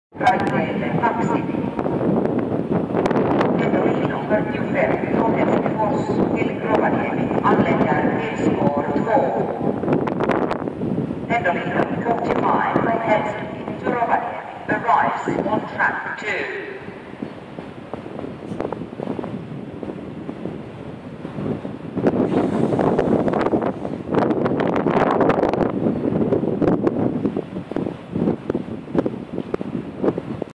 Blue skies at Tampereen rautatiesema
Blue skies and echoing announcements at Tampereen rautatiesema. Tampere, Finland